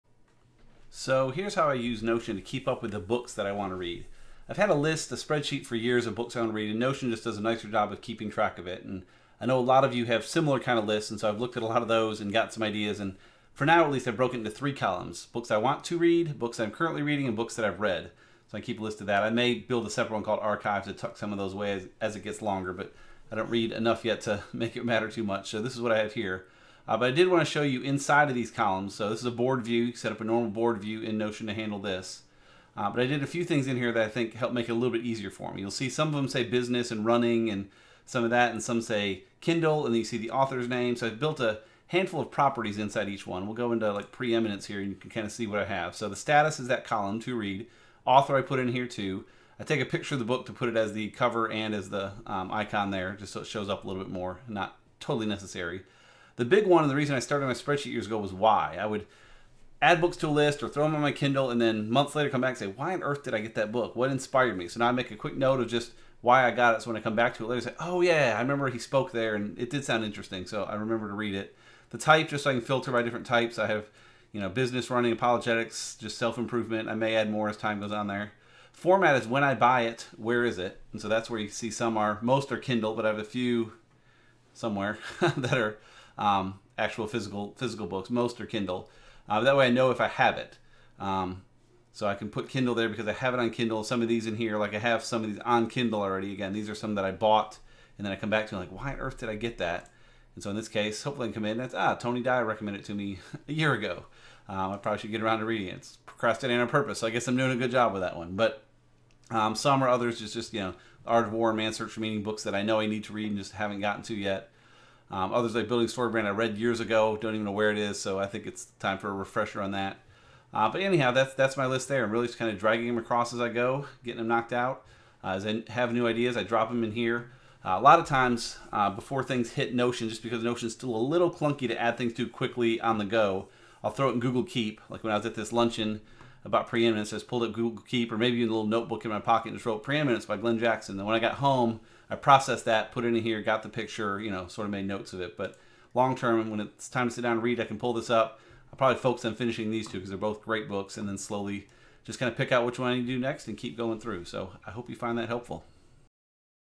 Keeping up with books to read using Notion
Most of us have a list of books that we want to eventually read. Using Notions database features, you can do some neat things to help make better sense of your list.